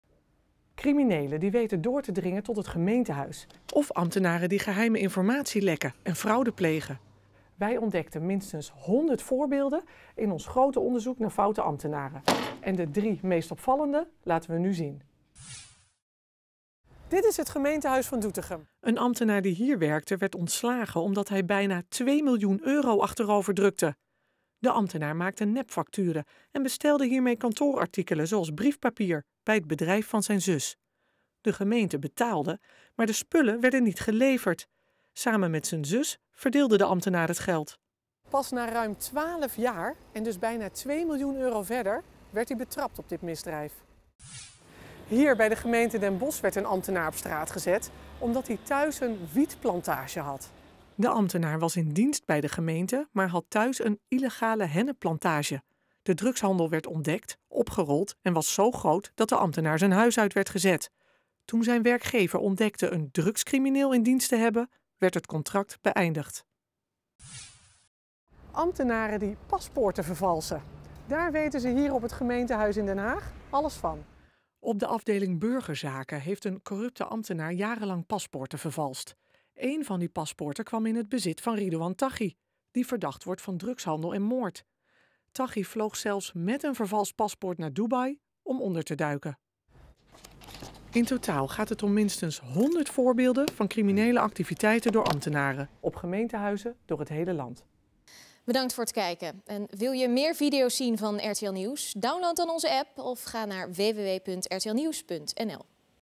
Criminelen infiltreren gemeentehuis tientallen keren per jaar
Criminelen dringen zeker tientallen keren per jaar door tot gemeentehuizen. Ze kopen ambtenaren om voor valse paspoorten, plegen fraude met spookfacturen of leggen de hand op geheime informatie. In deze video leggen we drie voorbeelden uit.